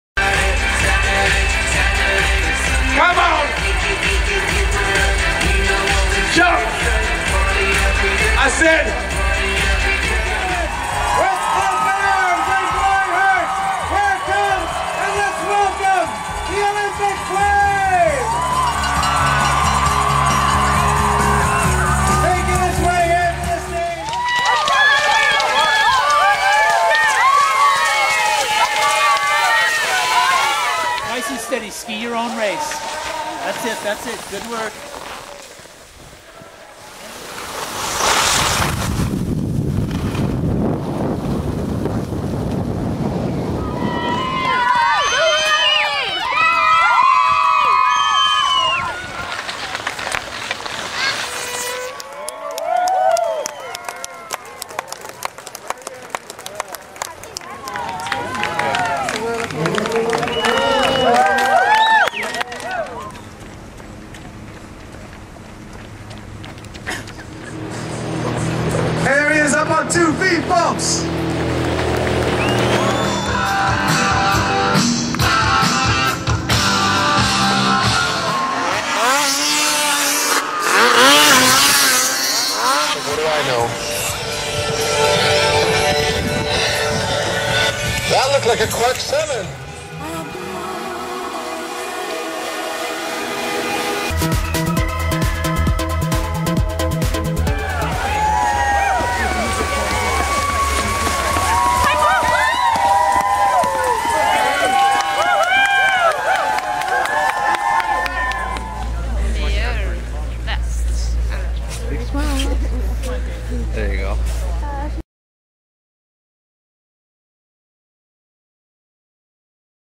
Final Gold Medal PlayLiveBeTobaccoFree.ca
Scenes to promote activities living a Tobacco Free lifestyle.  Final shot says it.  She won and is so French, her friends were so proud.  Think this shows activity has its rewards.